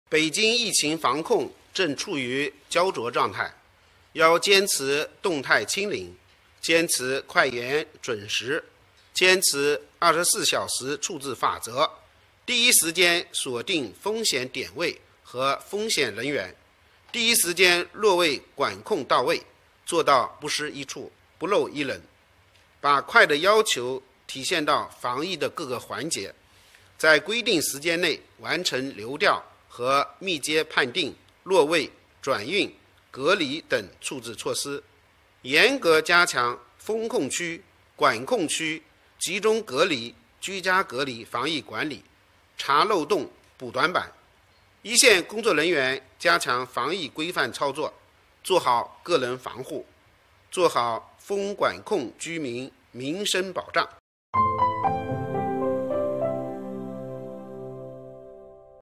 北京：要坚持“24小时处置法则” 做到不失一处 不漏一人
5月10日，在北京市新型冠状病毒肺炎疫情防控工作第330场发布会上，北京市委宣传部副部长徐和建表示，要坚持“24小时处置法则”，第一时间锁定风险点位和风险人员，第一时间落位管控到位，做到不失一处、不漏一人。把快的要求体现到防疫的各个环节，在规定时间内完成流调和密接判定、落位、转运、隔离等处置措施。
        徐和建强调，严格加强封控区、管控区、集中隔离、居家隔离防疫管理，查漏洞补短板，一线工作人员加强防疫规范操作，做好个人防护，做好封管控居民民生保障。